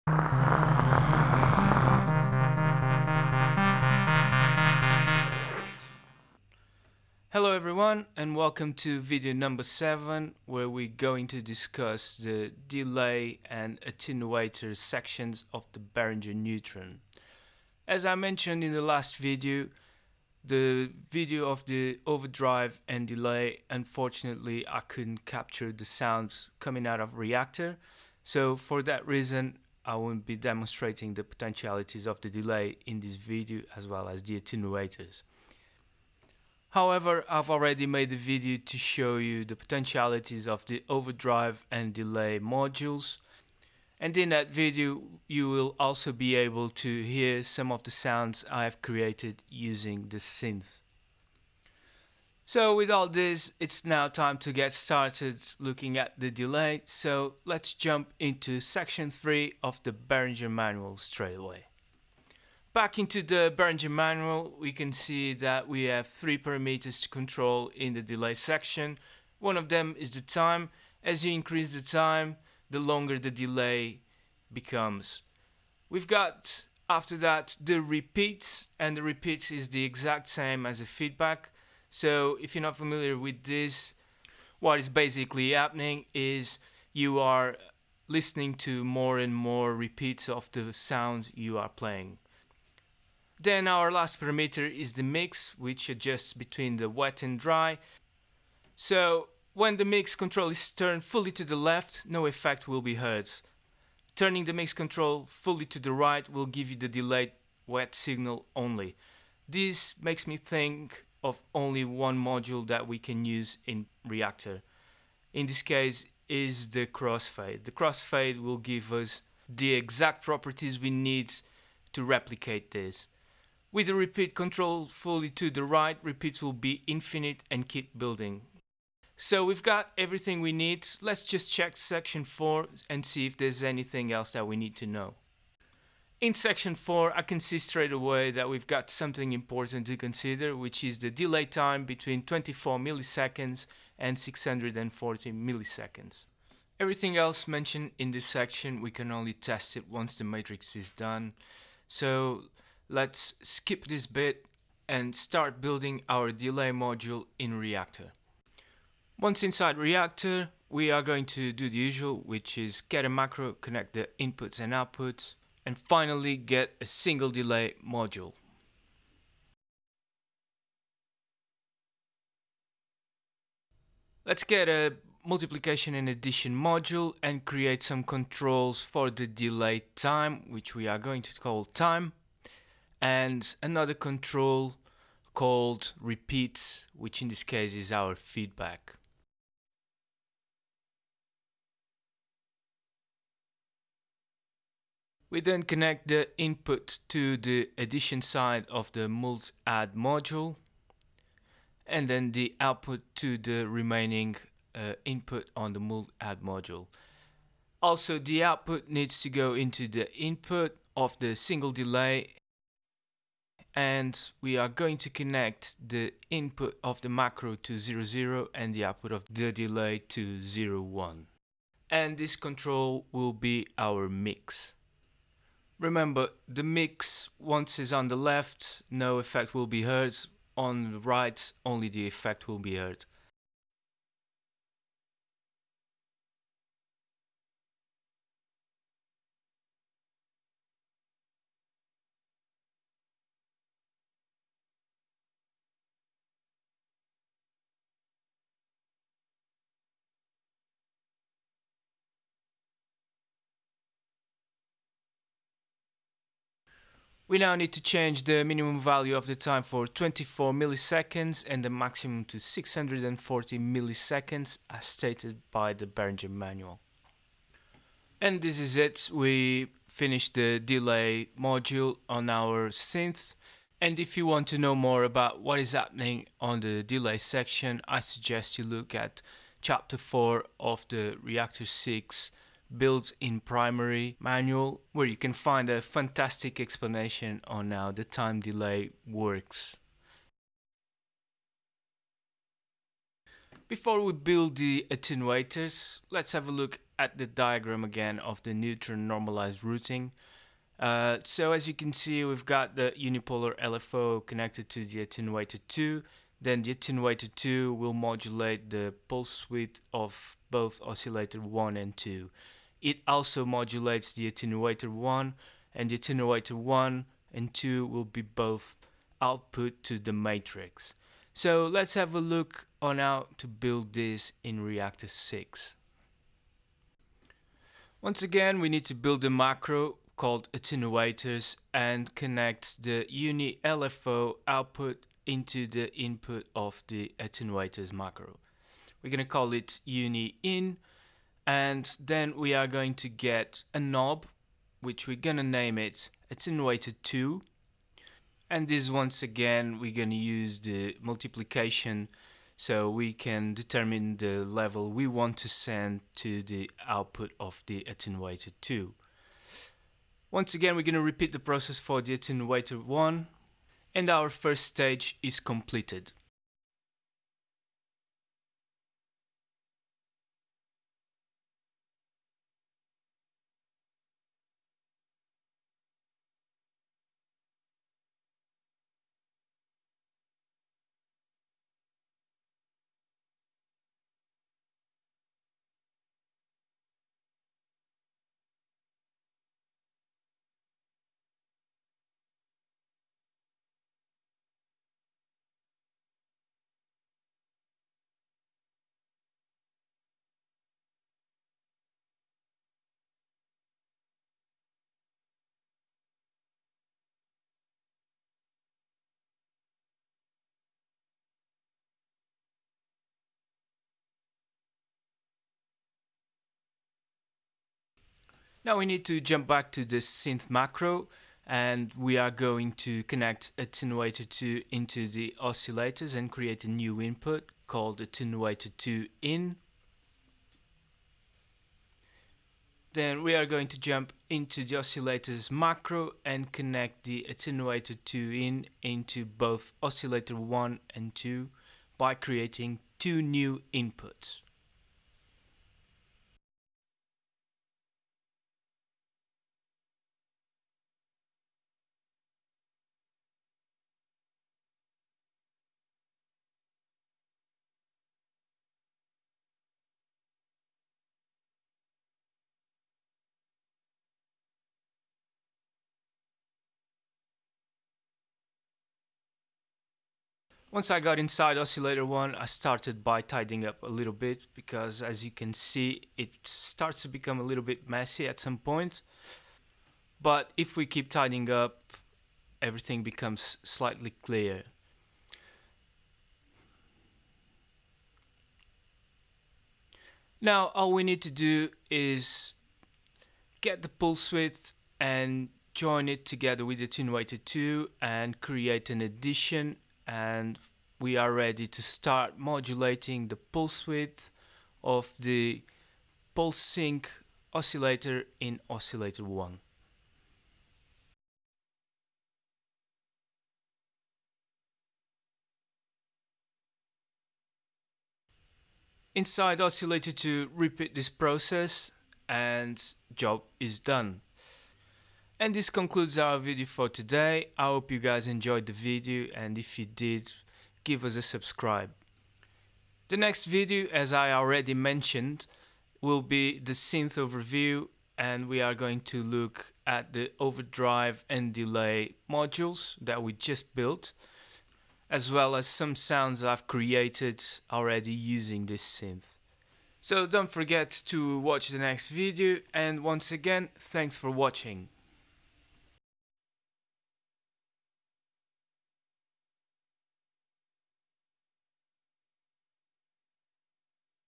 7 - Delay + Attenuators (Behringer Neutron into Reaktor 6 Tutorial)
Hello everyone. Today we are going to discuss both Delay an Attenuators modules in the Behringer Neutron and replicate it in Reaktor 6. Once again, apologies for not having sound coming out of Reaktor but I promise the next video will make up for that mistake. :)

00:00 Jingle
00:06 Introduction
01:02 Delay (Section 3 Behringer Manual)
02:15 Delay (Section 4 Behringer Manual)
02:38 Delay Module In Reaktor
04:35 Delay (Chpater 4 Reaktor 6 Building in Primary Manual)
04:53 Behringer Neutron Normalised Routing Diagram
05:27 Attenuators in Reaktor 6
06:58 Attenuator 2 into PWM 1&2